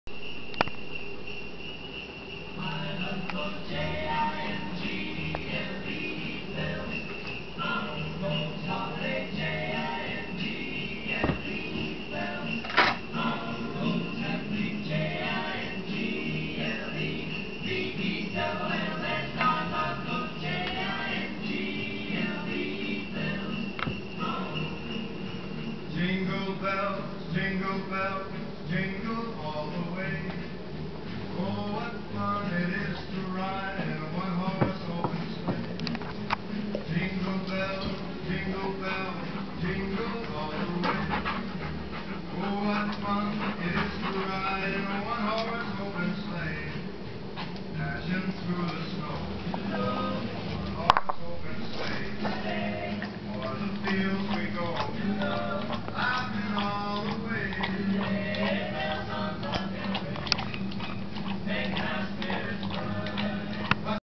Akita and Eggnog 1
here our festive holiday akita eagerly eats her first eggnog, all organic of course, of the year. Wish we could provide the handgrated nutmeg visuals and sound effects for you, but couldn't figure out how to do that and hold that camera all with 2 hands...part one